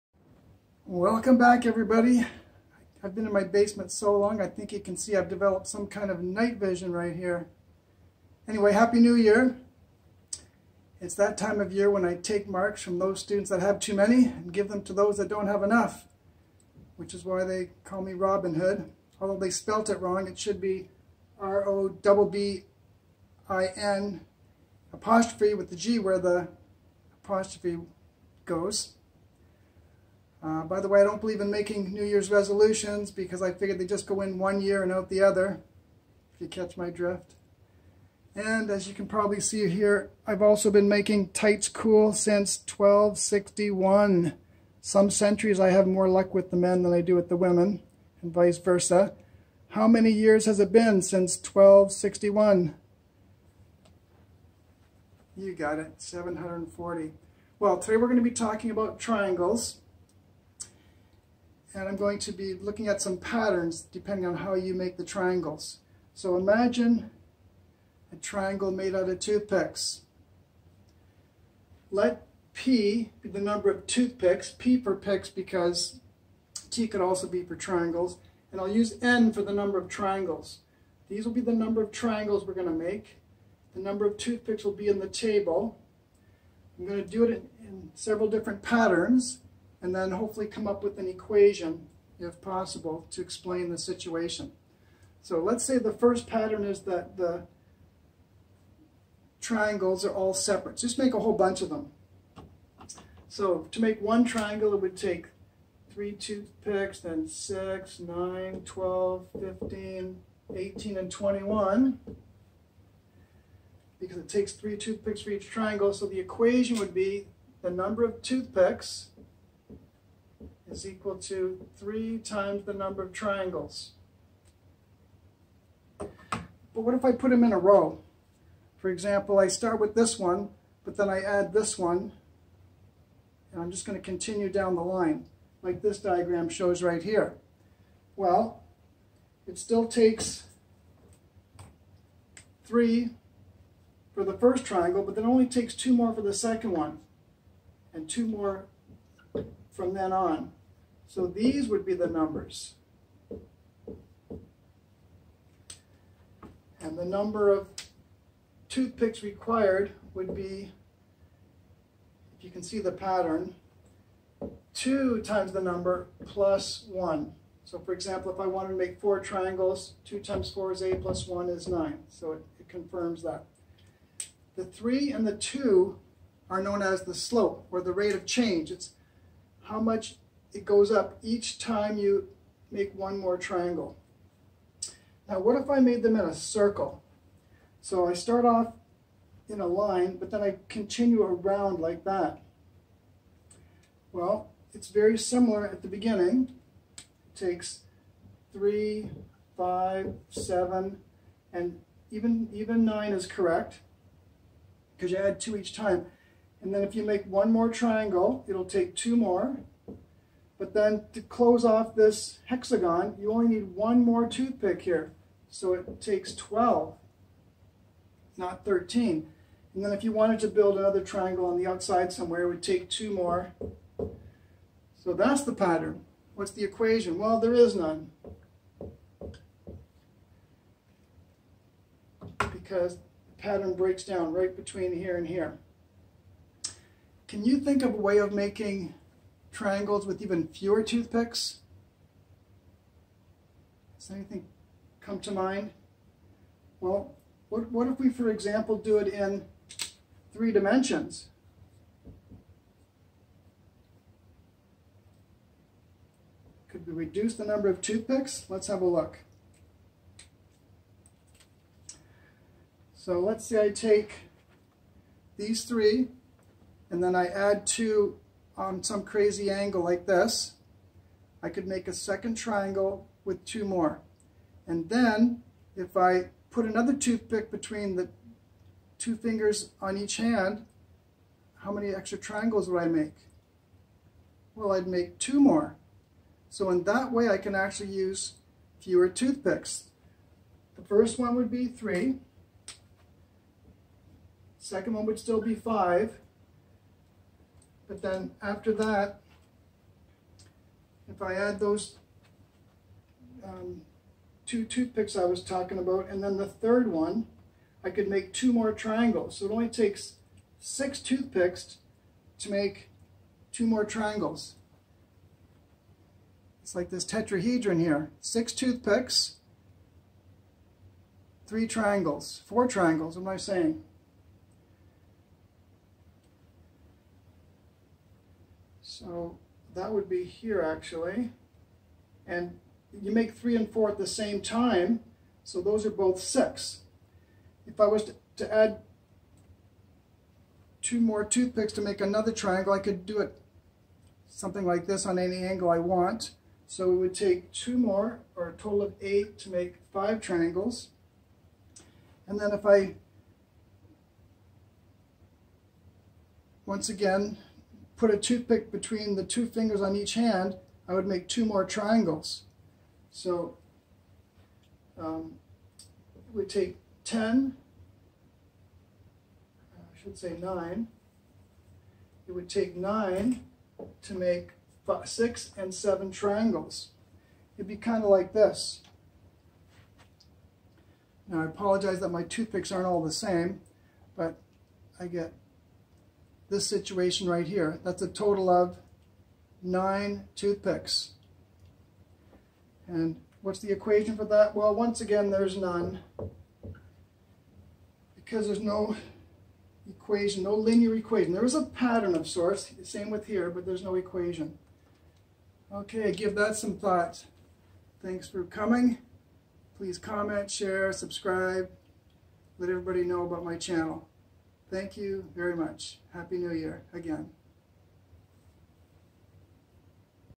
Triangulation (strangulation?!)
Mr. John Hush investigates patterns in drawing triangles in various ways. It's not as easy as first meets the eye. He maximizes the number of triangles created all the while minimizing the number of sides. Oops, he said 740 when he meant to say 760!